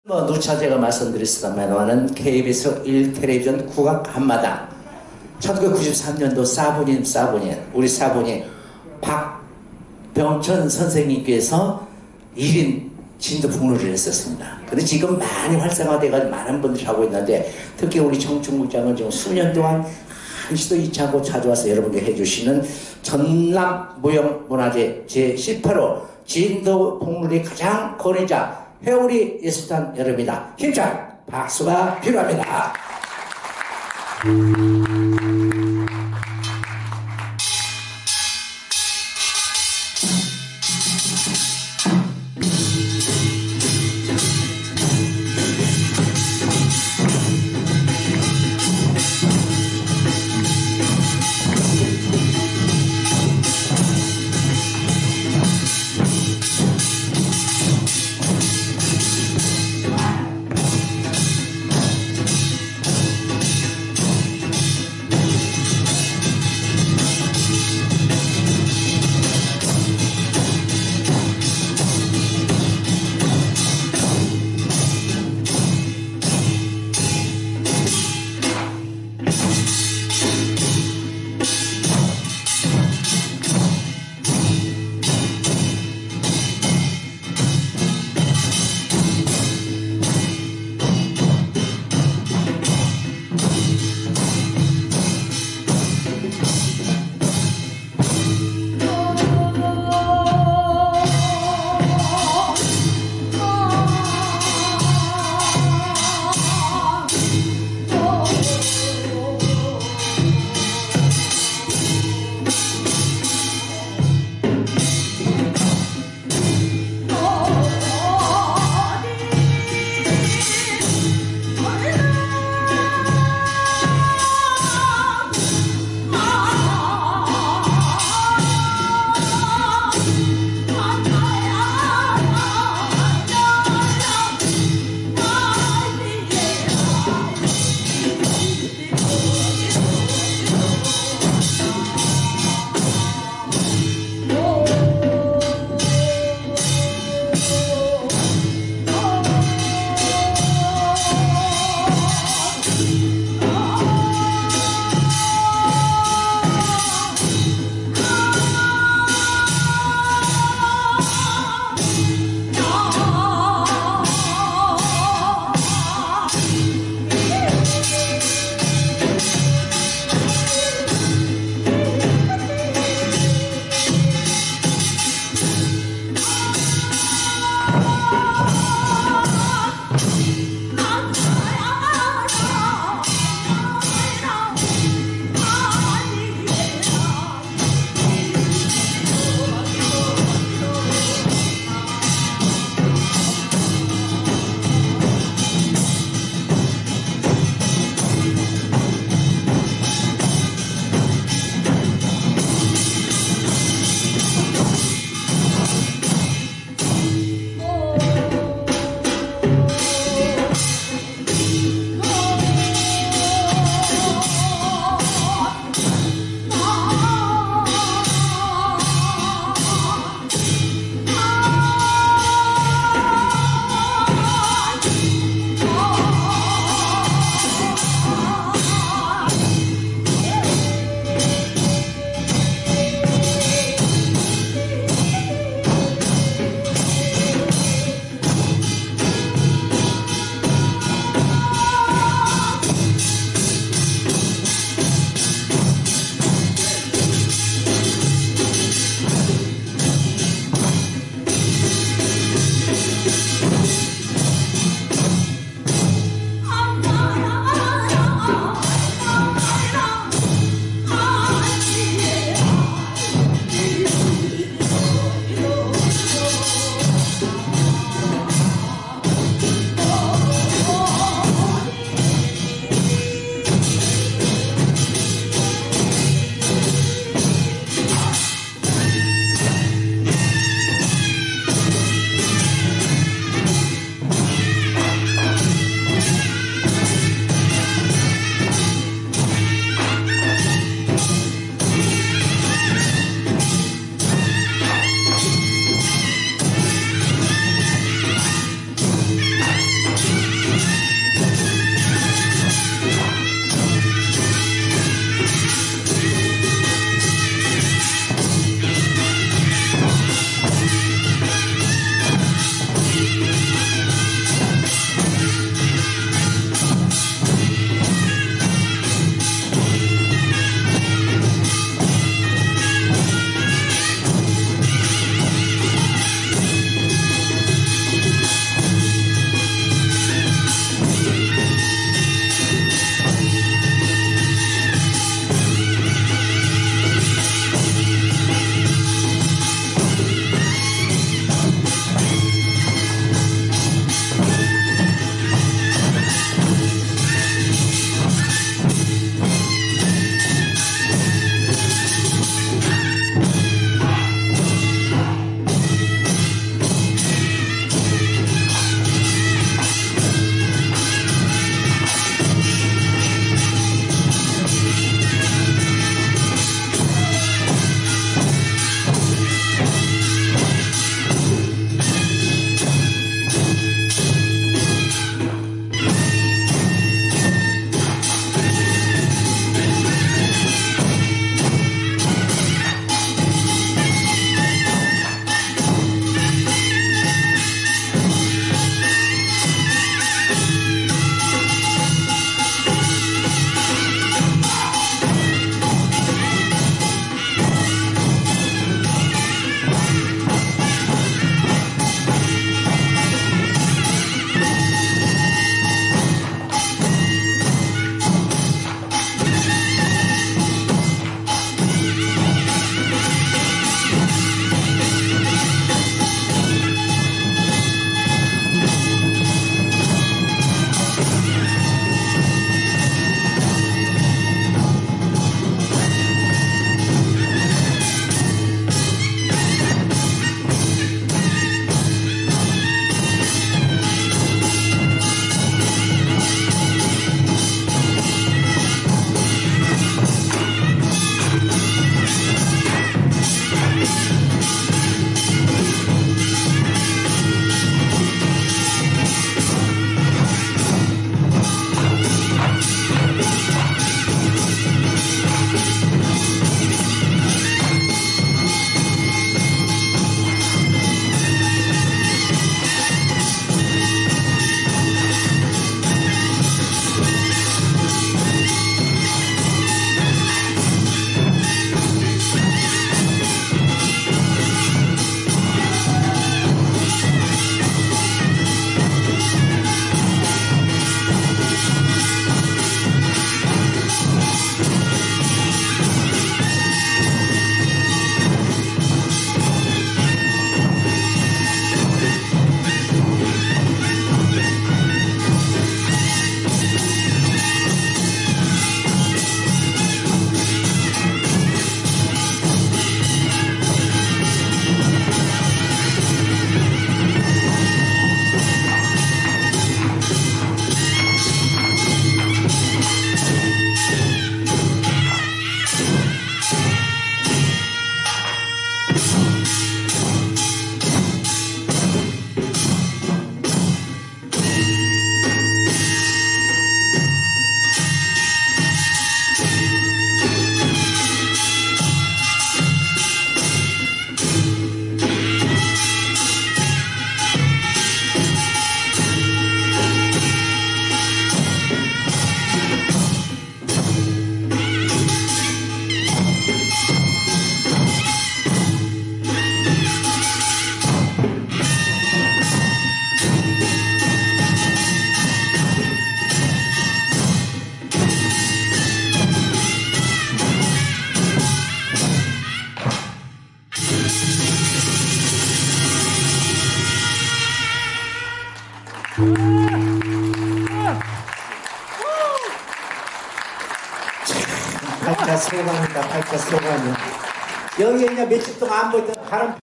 "진도 북놀이, 전남 무형 문화재 제18호 해우리 예술단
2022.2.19
어르신 문화 공연